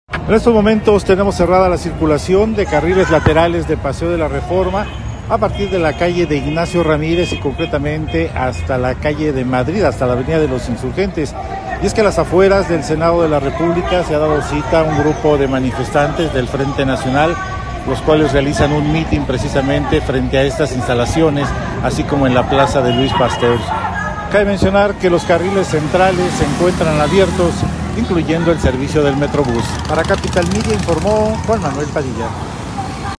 Manifestantes del Frente Nacional realiza un mitin frente al Senado de la República
ReporteVial | Un grupo de manifestantes realiza un mitin frente al Senado de la República entre la calle de Madrid y la avenida de los Insurgentes.